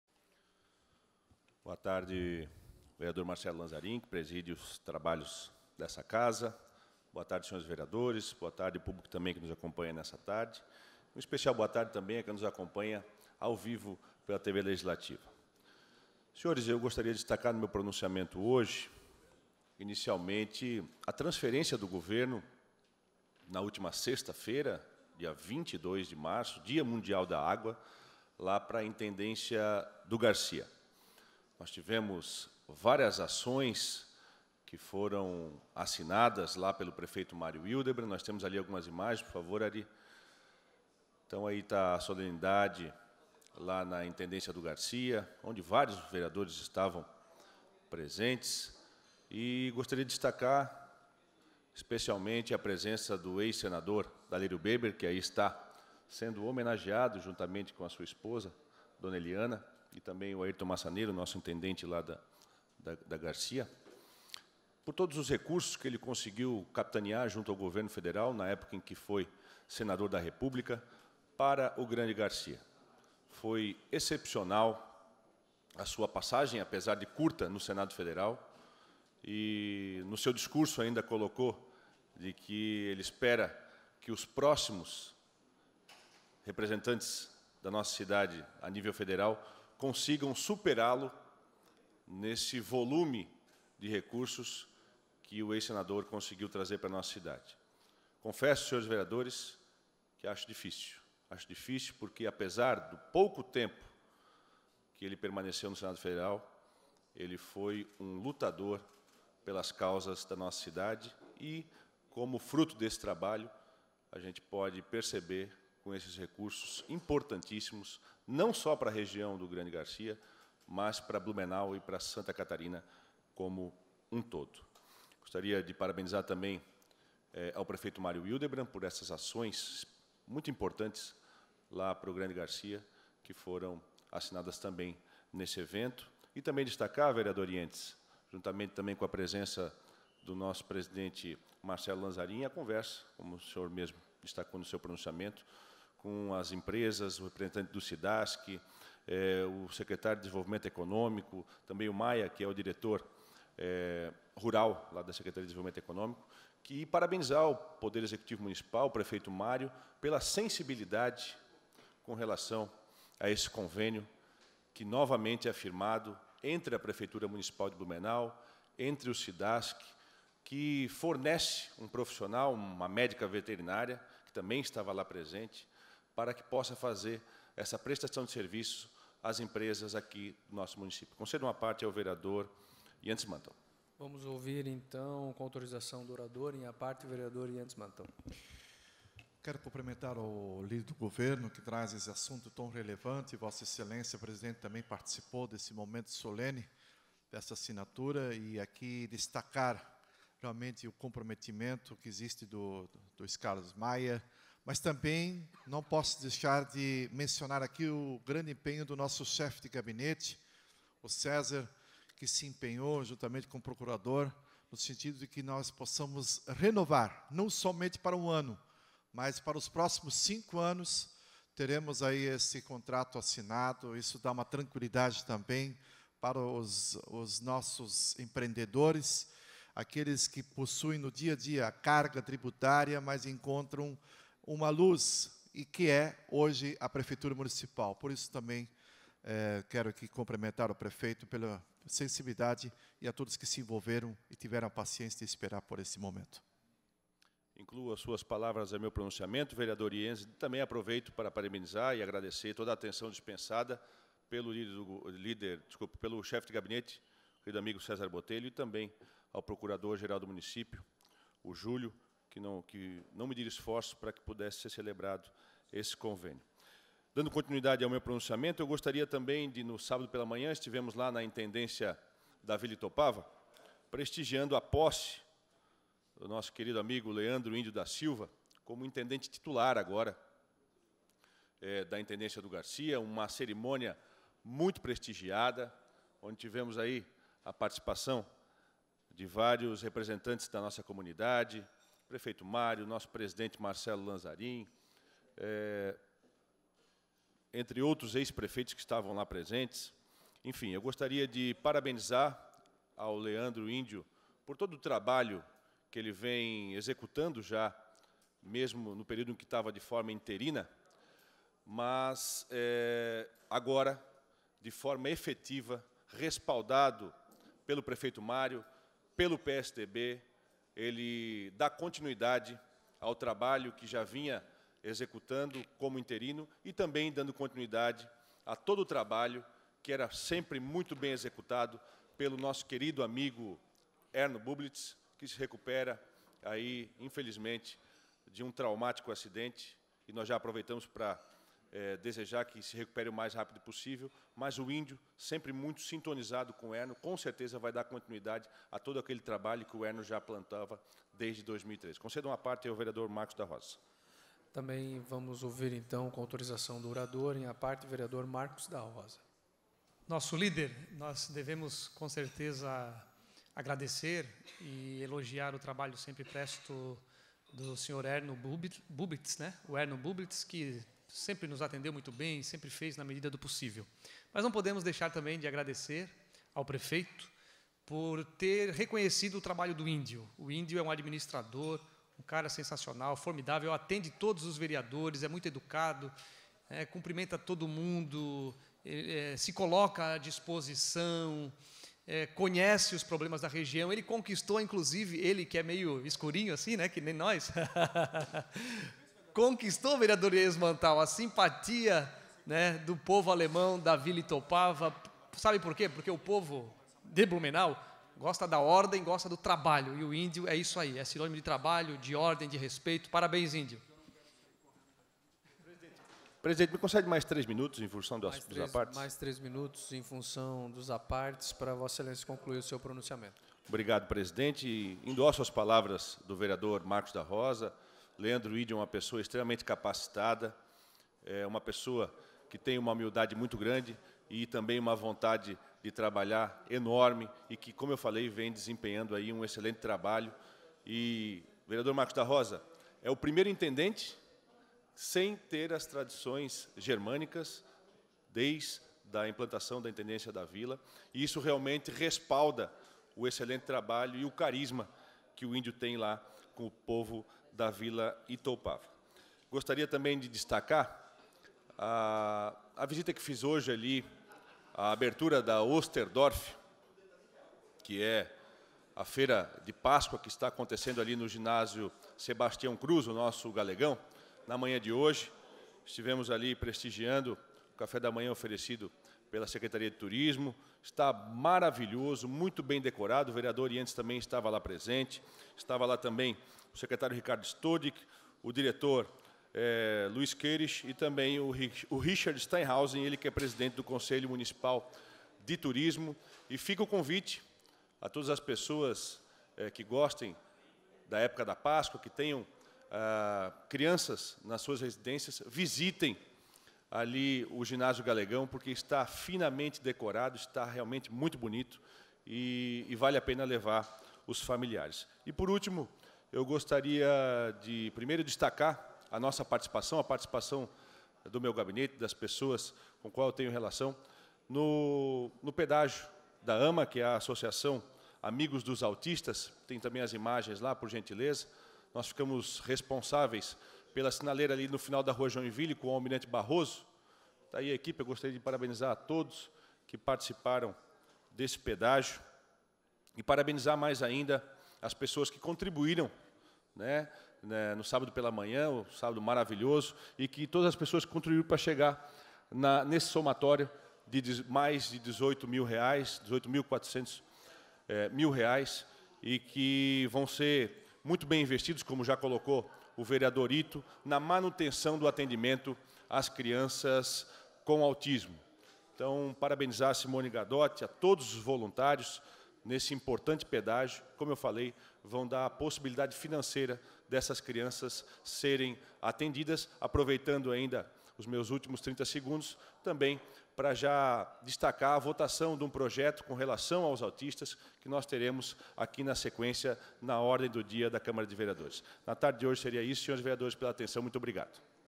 Pronunciamento Vereador Alexandre Matias - 26/03/2019
Vereador Alexandre Matias destaca eventos do Executivo na região Sul e Norte da cidade
O vereador e líder do governo Alexandre Matias (PSDB) falou da transferência simbólica do Executivo, no dia 22 de março, para a Intendência do Garcia. Destacou a assinatura de vários documentos realizados na cerimônia para viabilizar melhorias ao bairro do Garcia. Falou dos recursos que virão através do ex-senador Dalírio Beber (PSDB). Mencionou que apesar do pequeno tempo que foi representante da cidade em Brasília, trouxe recursos importantíssimos para Santa Catarina. Registrou ainda a sua presença na Intendência da Vila Itoupava para a cerimônia de posse do novo intendente o Leandro “Índio” da Silva (PSDB). Parabenizou aquele servidor que, já de forma interina, realizava um trabalho muito importante para aquela região. Lembrou ainda do ótimo trabalho feito anteriormente naquela região pelo servidor Erno Bublitz, que foi intendente e que se recupera de um acidente doméstico. Registrou sua participação na abertura da Osterdorf na manhã desta terça-feira (26). Ressaltou a beleza do espaço “finamente decorado” e convidou a população a prestigiar o evento que antecede a páscoa na cidade. Por fim, registrou o pedágio da AMA - Associação de Amigos do Autista realizado no último fim de semana e a arrecadação de cerca de R$ 18 mil.